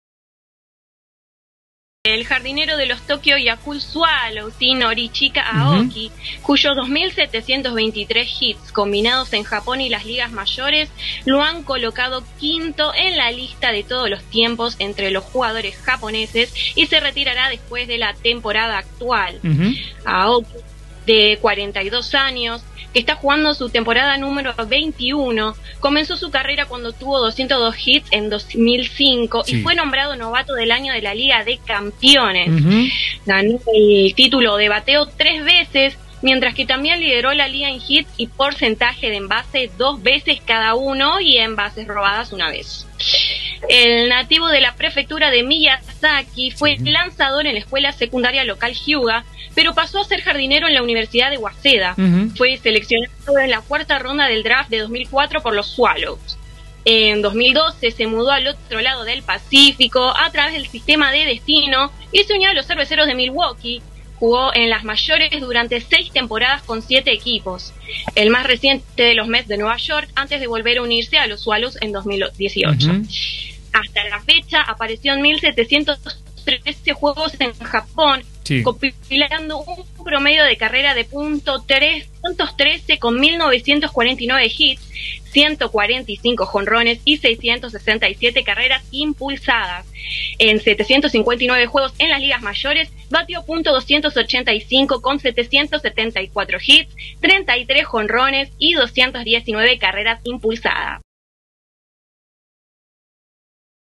NORICHIKA AOKI SE RETIRA DEL BÉISBOL PROFESIONAL - JH 18.09.24 / JAPÓN HOY TV
EL JAPONÉS NORICHIKA AOKI SE RETIRA DEL BEISBOL PROFESIONAL
  El jardinero de los Tokyo Yakult Swallows, Norichika Aoki, cuyos 2.723 hits combinados en Japón y las ligas mayores lo han colocando quinto en la lista de todos los tiempos entre los jugadores japoneses, se retirará después de la temporada actual.
  Aoki, de 42 años, que está jugando su temporada número 21, comenzó su carrera con fuerza cuando tuvo 202 hits en 2005 y fue nombrado Novato del Año de la Liga de Campeones. Ganó el título de bateo tres veces, mientras que también lideró la liga en hits y porcentaje de embase dos veces cada uno, y en bases robadas una vez.
  El nativo de la prefectura de Miyazaki fue lanzador en la escuela secundaria local Hyuga, pero pasó a ser jardinero en la Universidad de Waseda. Fue seleccionado en la cuarta ronda del draft de 2004 por los Swallows.
  En 2012, se mudó al otro lado del Pacífico a través del sistema de destino y se unió a los Cerveceros de Milwaukee. Jugó en las mayores durante seis temporadas con siete equipos, el más reciente de los Mets de Nueva York, antes de volver a unirse a los Swallows en 2018.
  Hasta la fecha, apareció en 1.713 juegos en Japón, compilando un promedio de carrera de .313 con 1.949 hits, 145 jonrones y 667 carreras impulsadas. En 759 juegos en las ligas mayores, bateó .285 con 774 hits, 33 jonrones y 219 carreras impulsadas.
  Esta temporada, Aoki, el jugador de posición de mayor edad en el béisbol profesional de Japón, ha aparecido en solo 61 juegos, bateando .192 sin jonrones y nueve carreras impulsadas.